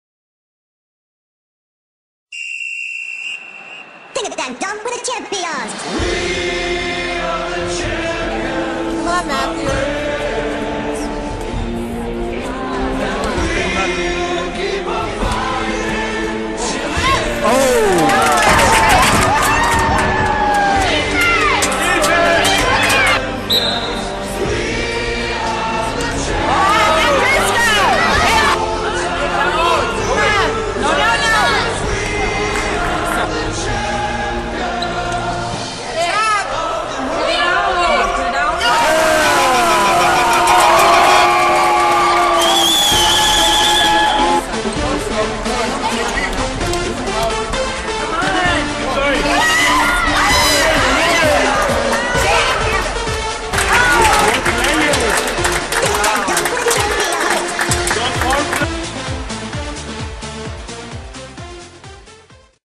FC U 12 Futsal Champions 2 15 14
Won in 2 overtime FC vs Croations